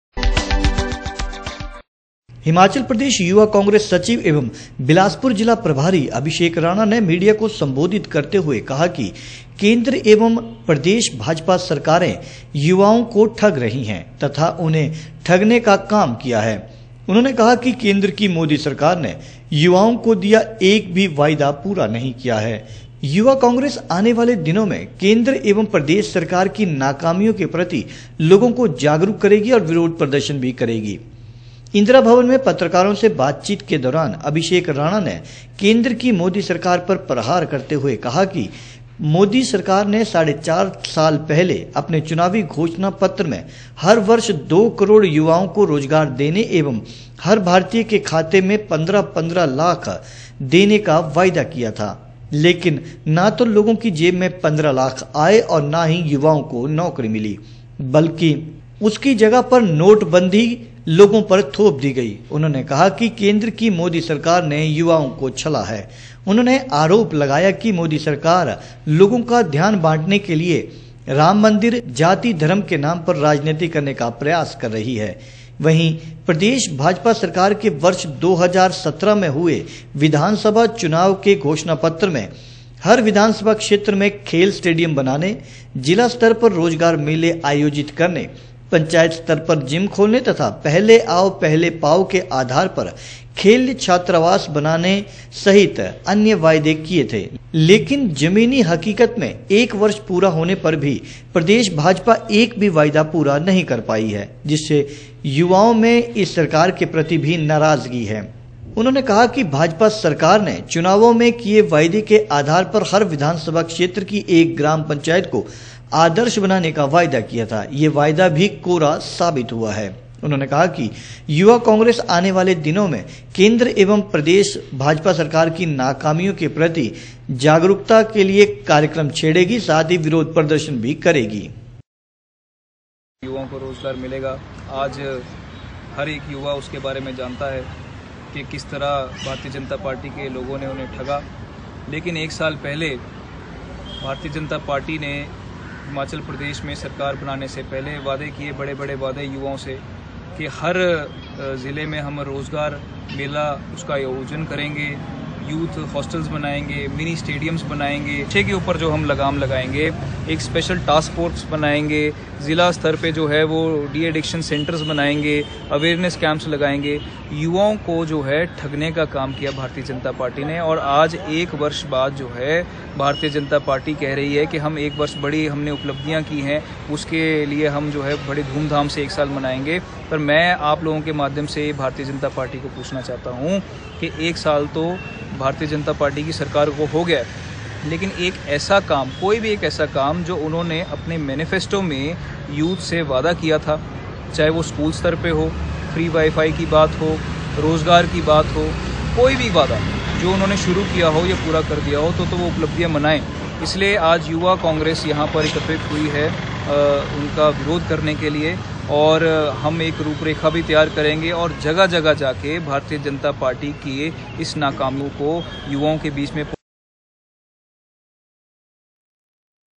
केंद्र एवं प्रदेश भाजपा सरकार ने युवाओं ने अब तब तक ठगा कहा अभिषेक राणा ने
हिमाचल प्रदेश युवा कांग्रेस सचिव एवं बिलासपुर जिला प्रभारी अभिषेक राणा ने मीडिया को सम्बोधित करते हुए  कहा कि केंद्र एवं प्रदेश भाजपा सरकार ने युवाओं ने अब तब तक ठगा है। तथा उन्हें ठगने का काम किया है। केंद्र की मोदी सरकार ने युवाओं के साथ किए गए वादे को पूरा नहीं किया है। युवा कांग्रेस आने वाले दिनों में केंद्र एवं प्रदेश भाजपा सरकार की नाकामियों के प्रति लोगों को जागरूक करेगी व विरोध प्रदर्शन भी करेगी। वह शुक्रवार को यहां पर इंदिरा भवन में पत्रकारों को संबोधित कर रहे थे।